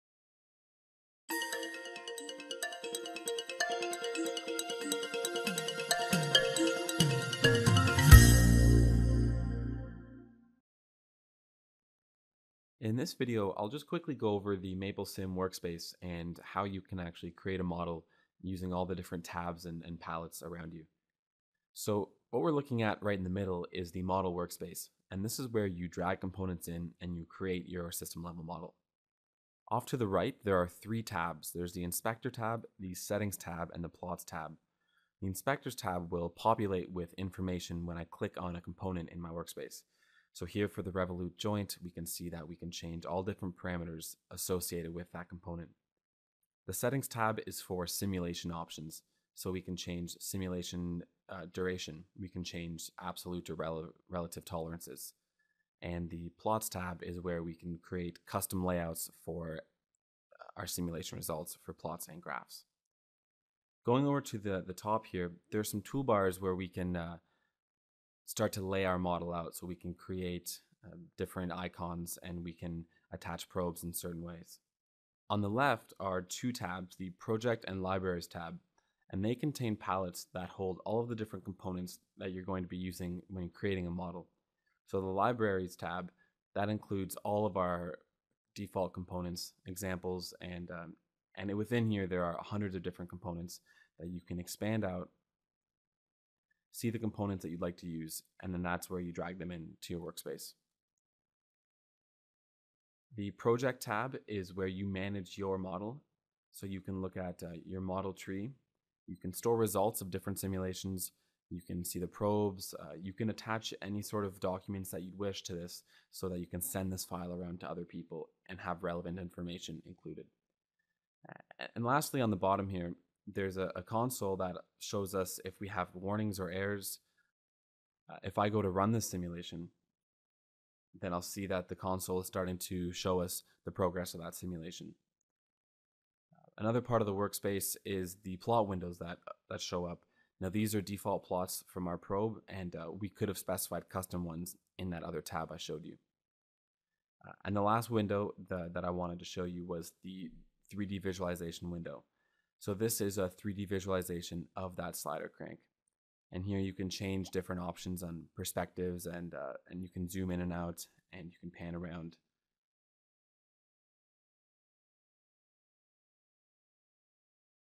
MapleSim Tutorial: The MapleSim Workspace
View an introduction to the various toolbars, palettes, and panes of the MapleSim workspace.